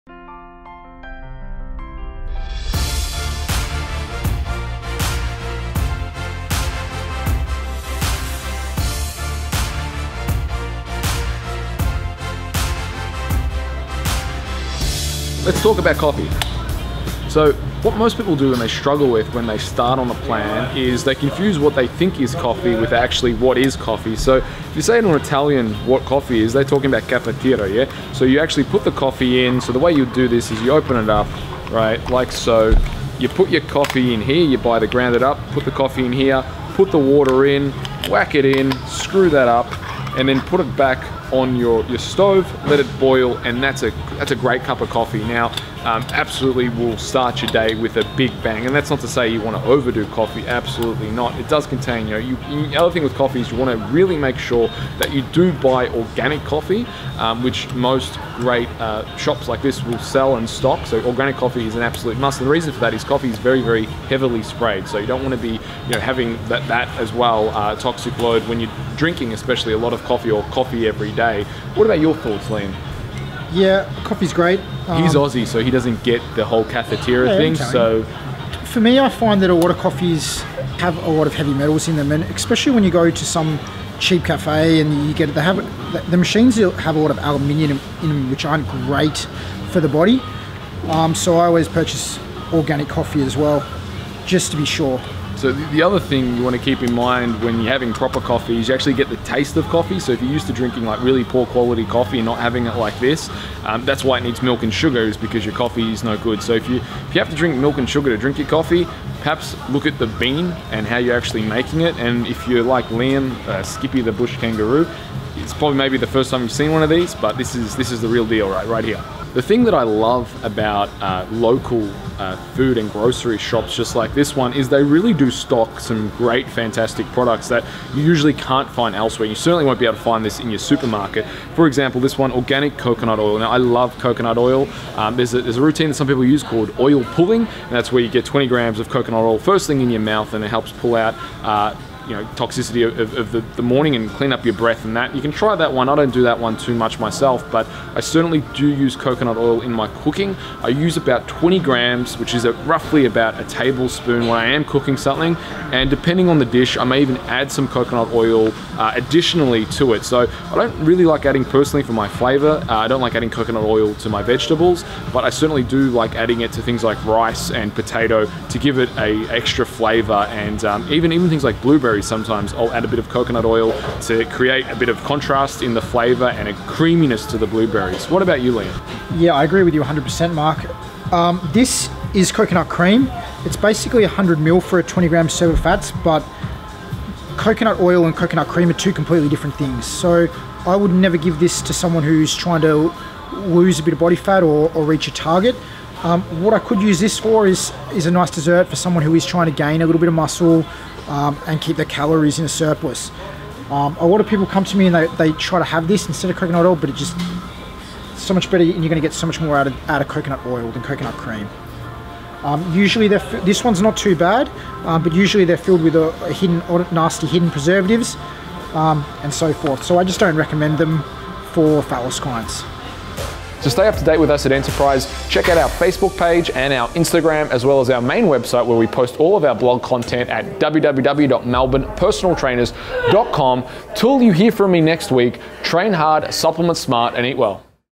How to Buy Good Coffee and Coconut Oil
ARE YOU A PERSONAL TRAINER AND NEED A HAND?

CONNECT WITH ENTERPRISE FITNESS

Mark and Liam visit Coltivare in Camberwell and review buying good Coffee and Coconut Oil. With coffee, you want to make sure you buy organic! Coconut oil, always opt for high-quality extra virgin!